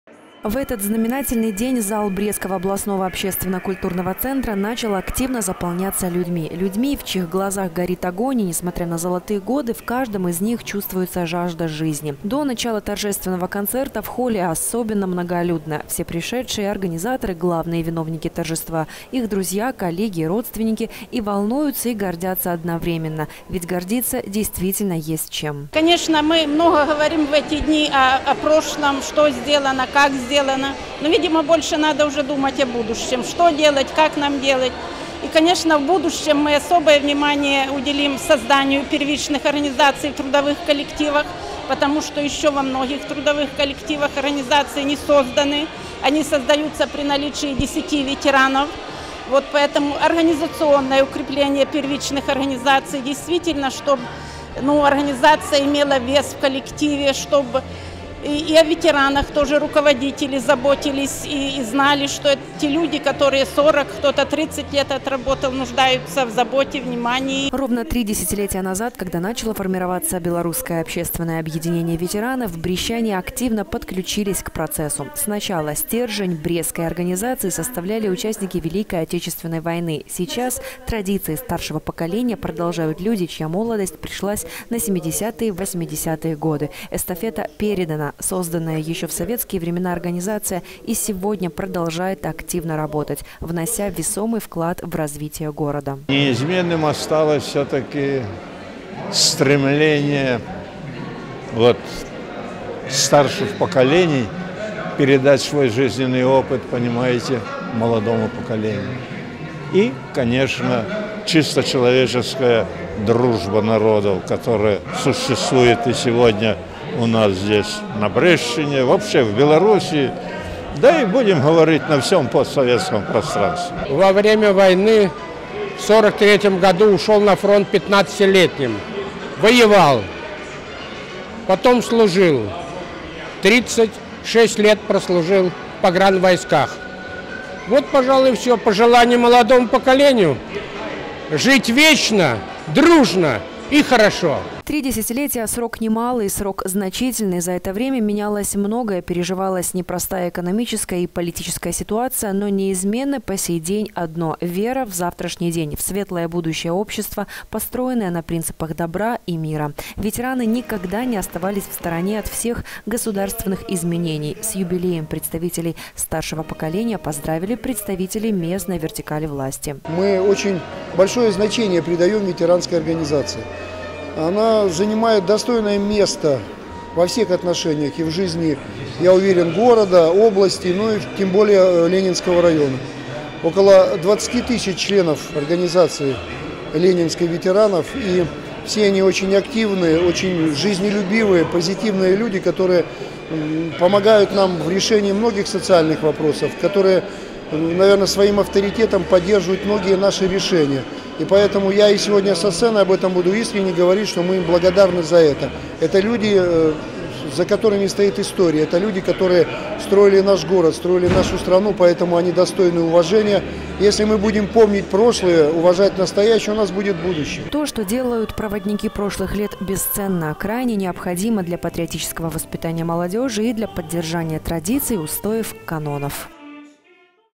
2017-03-07 г. Брест. Спецрепортаж: 30 лет Совету ветеранов Ленинского района Новости на Буг-ТВ.
Они – гарант мудрости и образец порядочности, их годы точно прожиты не зря. 30 лет назад в республике было создано Белорусское общественное объединение ветеранов. В него входят участники Великой Отечественной войны, Герои Советского Союза, Герои Социалистического труда, узники фашистских концлагерей, блокадники Ленинграда, воины-интернационалисты. Праздничные концерты и тематические встречи, приуроченные к этой дате, прошли во всех уголках Синеокой. Торжественное мероприятие по случаю юбилея Совета ветеранов Ленинского района города над Бугом состоялось на днях в областном центре.